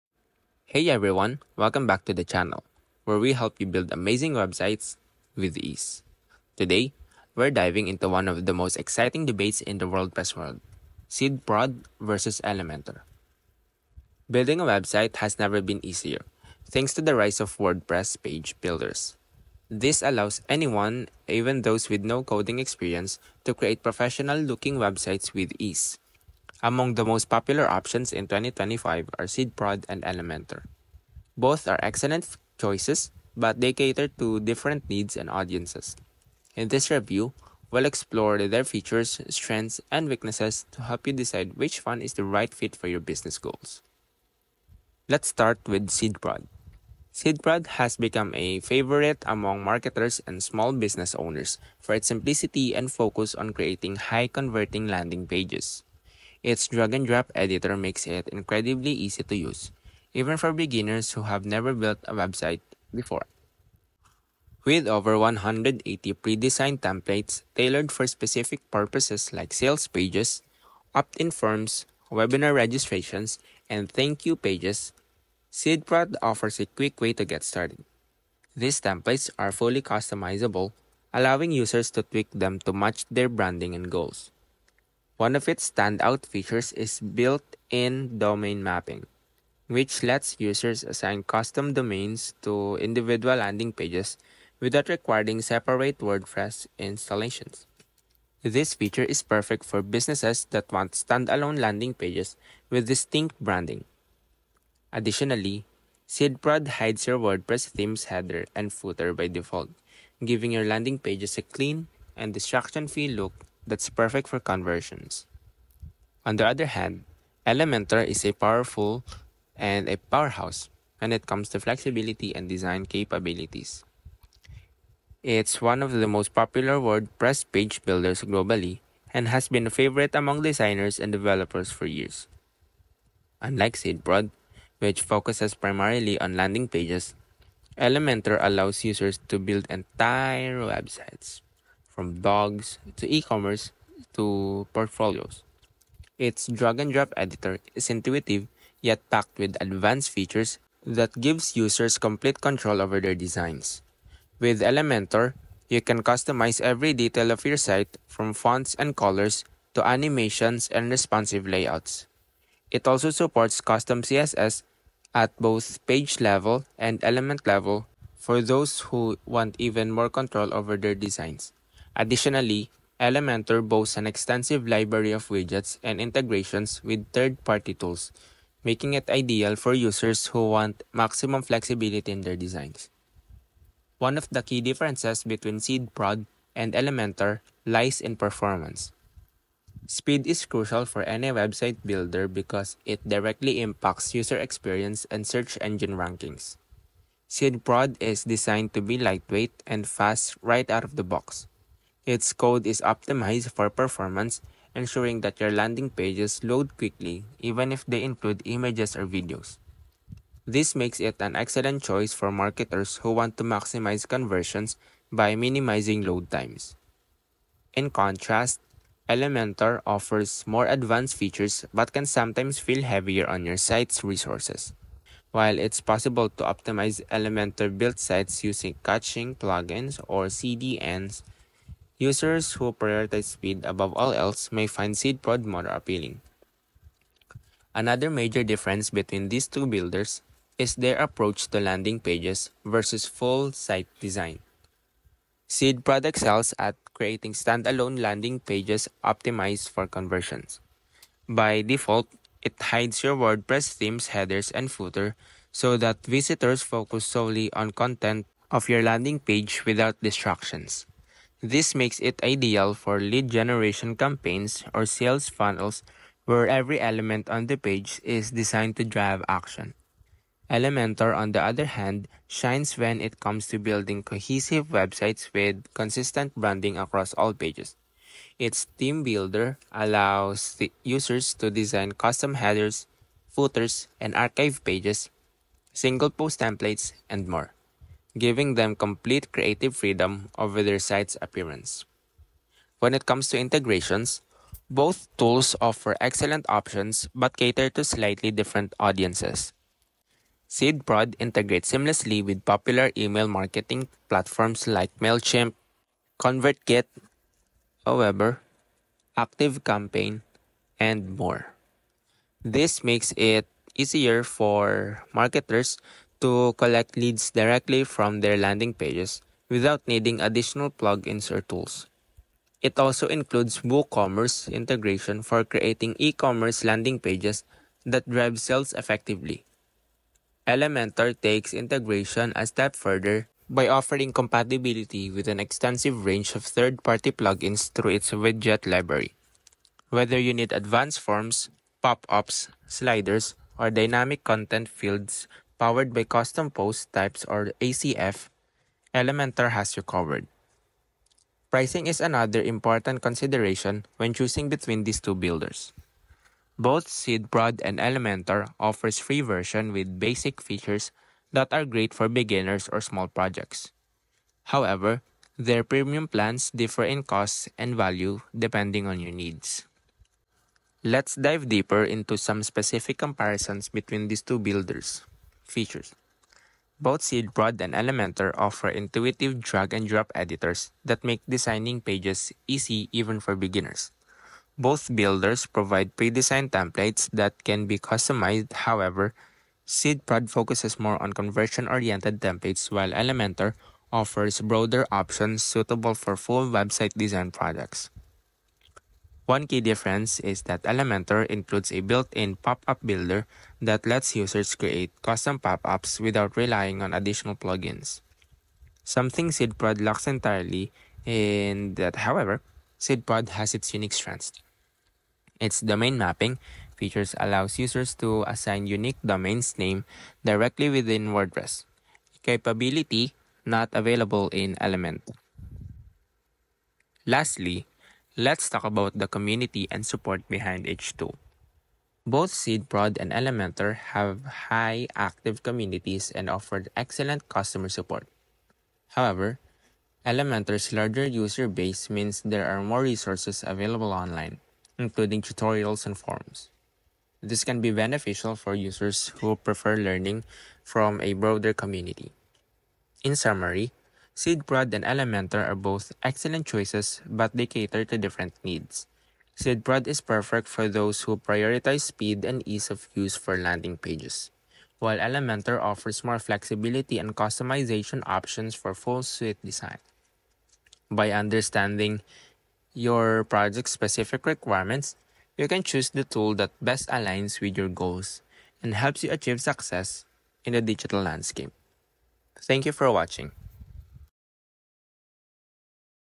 SEEDPROD VS ELEMENTOR: WHICH WORDPRESS PAGE BUILDER IS BETTER IN 2025?
‎ ‎ In the ever-evolving landscape of WordPress page builders, SeedProd and Elementor have emerged as prominent contenders. This video provides an in-depth comparison of both tools as of 2025, evaluating their features, performance, ease of use, and suitability for various website projects. Whether you're a beginner seeking simplicity or a developer desiring extensive customization, this analysis will assist you in determining which page builder aligns best with your needs.​